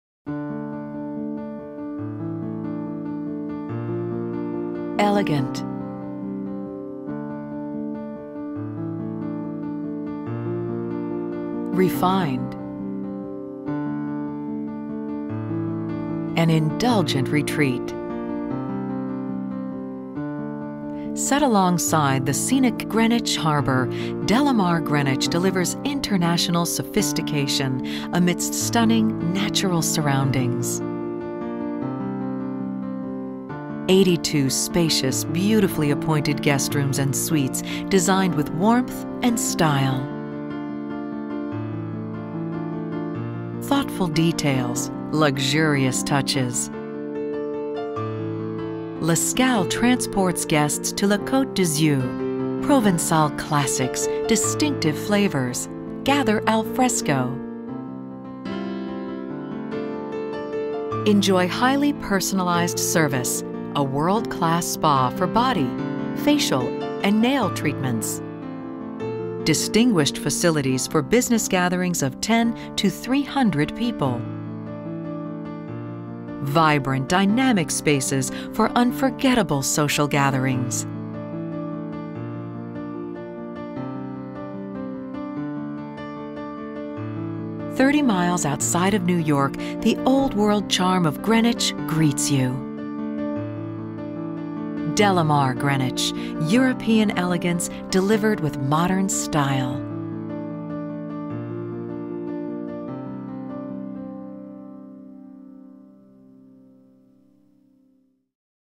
Delamar Greenwich HD H.264.mov
Located alongside the gorgeous Greenwich waterfront, Delamar Greenwich Harbor boasts 82 sumptuous rooms, an exquisite Spa, and l'escale restaurant and tavern, a local favorite serving exceptional Provincial cuisine.